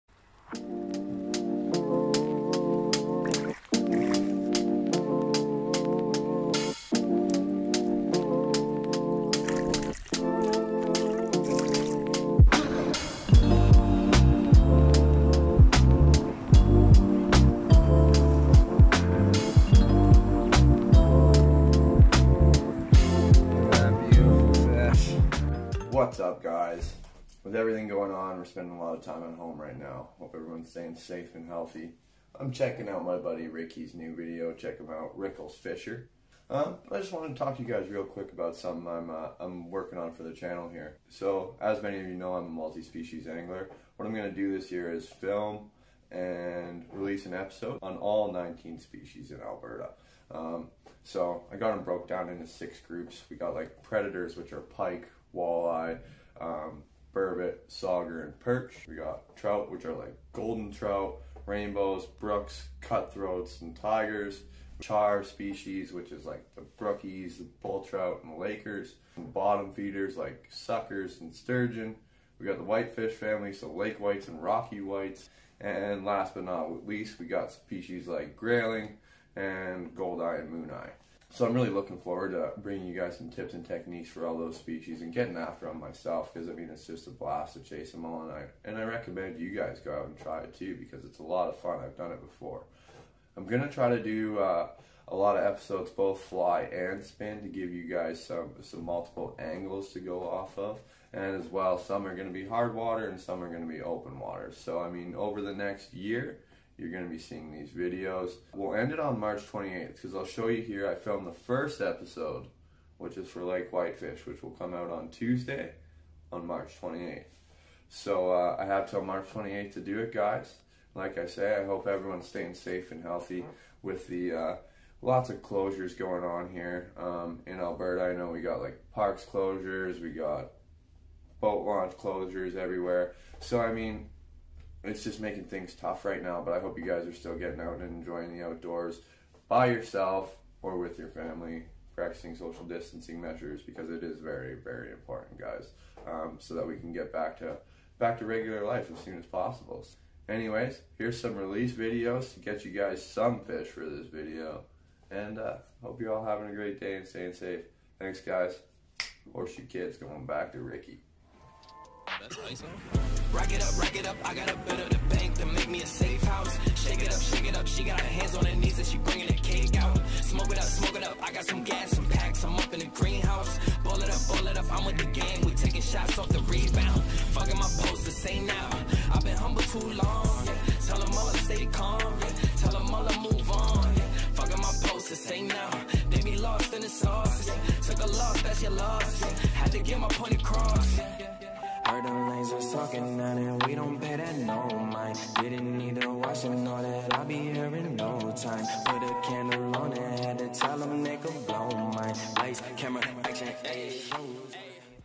19 Species Fishing Challenge! (Fly and Spin) Episode 1
Hey everyone!

Episode 1 of the 19 Species Fly and Spin Fishing Challenge.
On this episode I break down the species I'm going to target, the start date, and how I plan to bring it to you guys!

Super stoked on this as Alberta has some incredible species that take a multitude of skills and knowledge to be able to connect with.

Episode 2 drops Tuesday April 14th at 3pm!

🐟Species list:
Pike -
Walleye -
Sauger -
Perch -
Burbot -
Golden Trout -
Rainbow Trout -
Brown Trout -
Cutthroat Trout -
Tiger Trout -
Brook Trout -
Bull Trout -
Lake Trout -
Arctic Grayling -
Goldeye/Mooneye -
Sucker -
Sturgeon -
Lake Whitefish -
Rocky Mountain Whitefish -